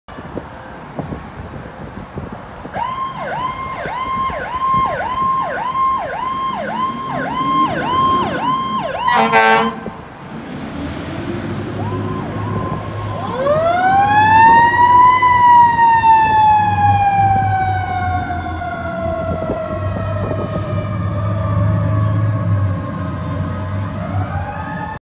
Montgomery County (RVFD3) Full House Response 1.11.19
Rockville Station 3 full house response to a reported apartment fire. Engine 703, Rescue Squad 703, and Tower 703.